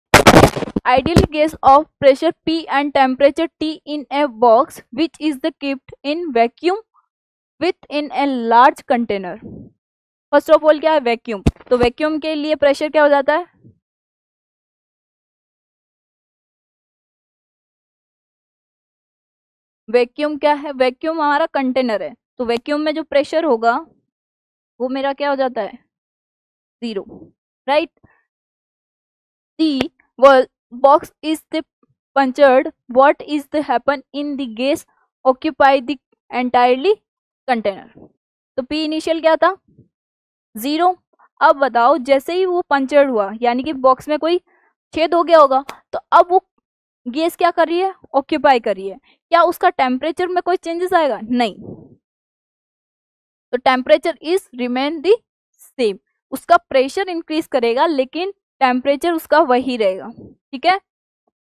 An ideal gas is at pressure P and temperature T in a box, which is kept in vacuum within a large....
An ideal gas is at pressure P and temperature T in a box, which is kept in vacuum within a large container. The wall of the box is punctured. What happens as the gas occupies the entire container?